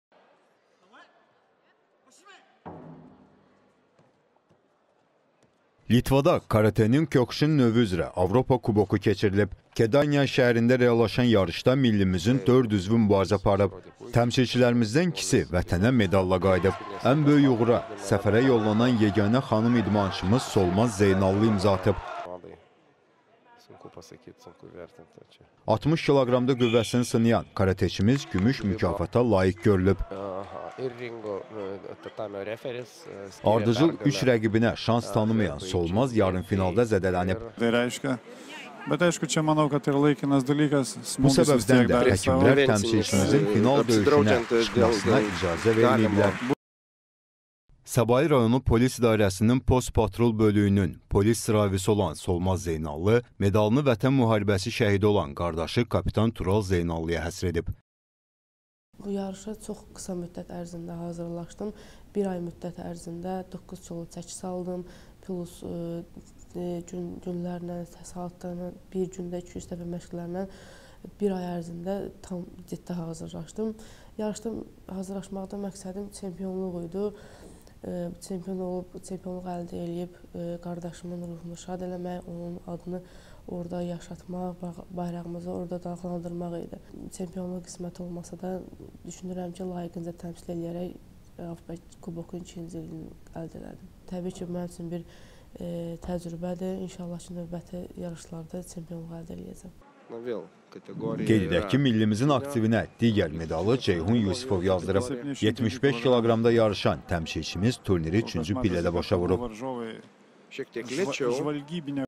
Millimiz vətənə iki medalla qayıdıb
Karateçilərimiz Avropa Kubokunda gümüş və bürünc mükafat qazanıblar